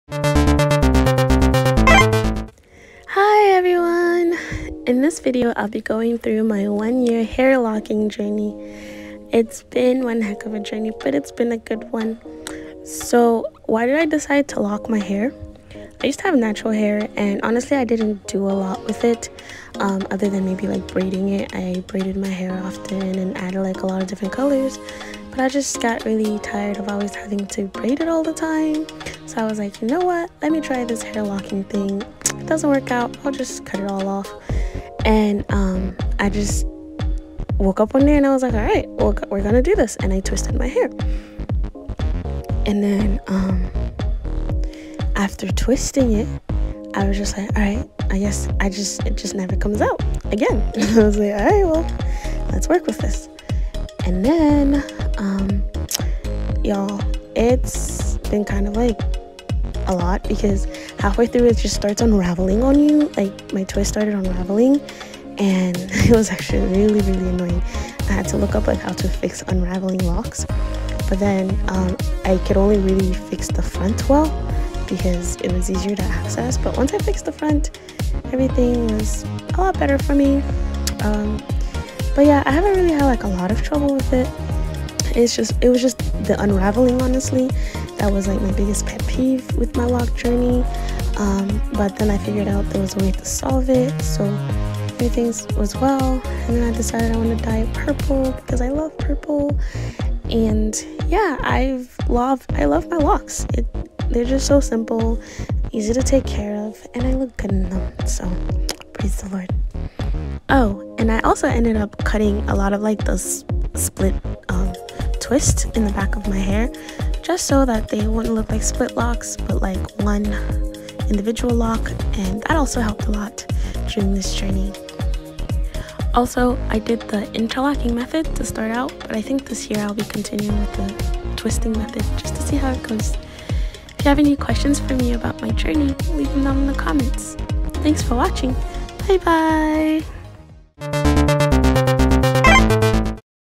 My Hair Locking Journey | 1 Year
It's been a year since I started locking my hair.
I'm very happy I took this path, and don't see myself turning back. I'm still learning a few things about it.